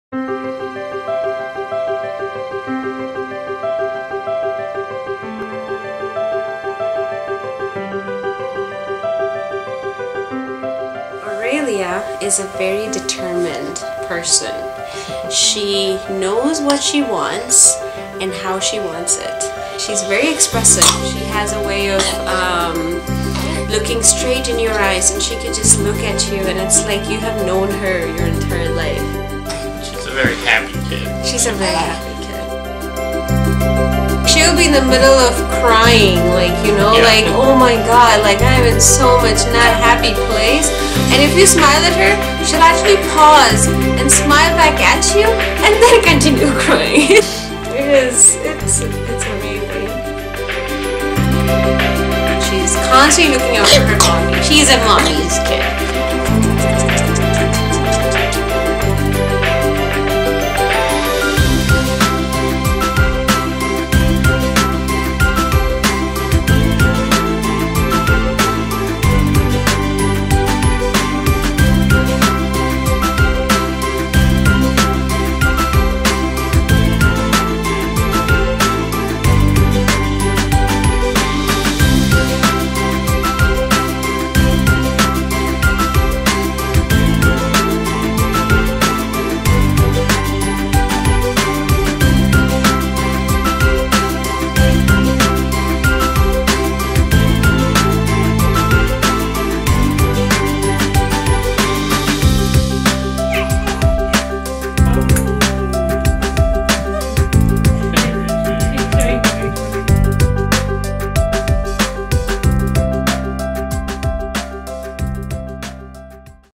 Baby Milestones: Aurelia
Silje Lynne Videography, A baby's 6 months Milestones. Music by: HookSound